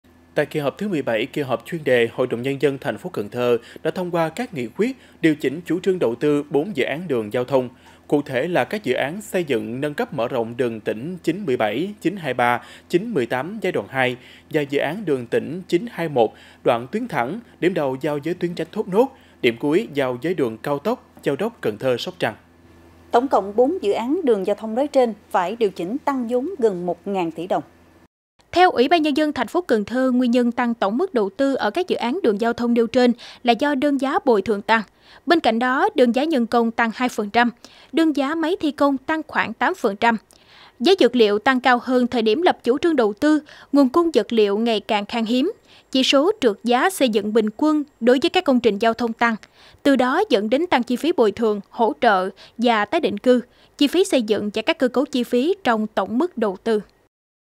4 dự án giao thông ở TP. Cần Thơ tăng vốn đầu tư gần 1.000 tỷ đồng | Cần Thơ TV
Tại kỳ họp thứ 17 (kỳ họp chuyên đề), HĐND TP Cần Thơ đã thông qua các nghị quyết điều chỉnh chủ trương đầu tư 4 dự án đường giao thông.